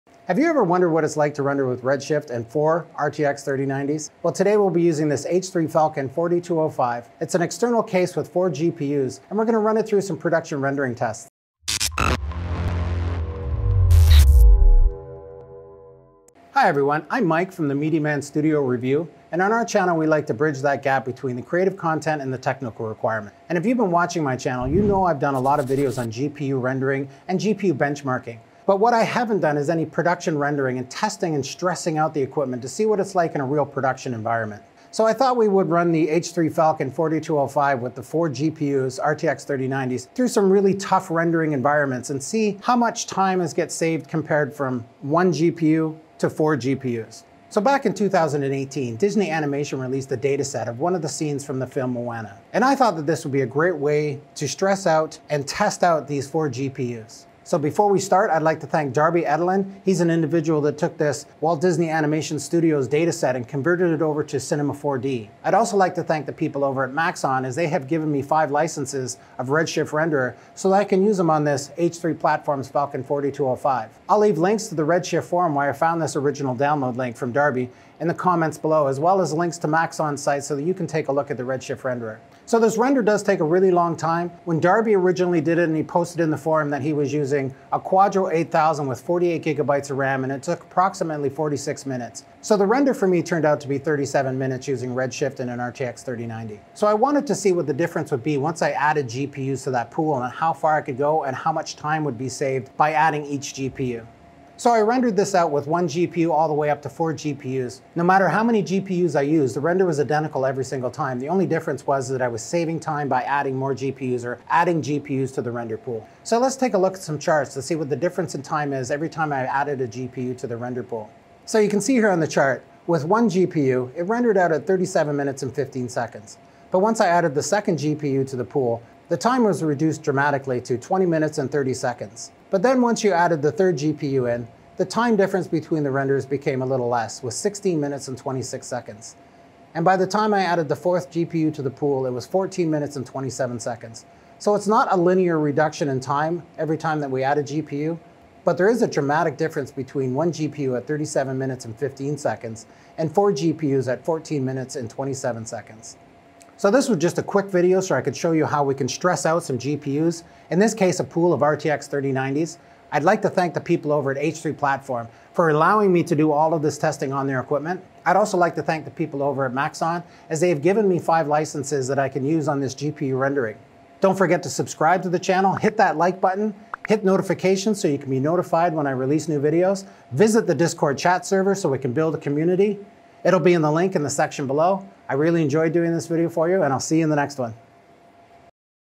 Production rendering 4 RTX3090s and Redshift/C4D
How many GPUs do you need for your production rendering?
Testing out 4 RTX3090 GPUs in production with Redshift and C4D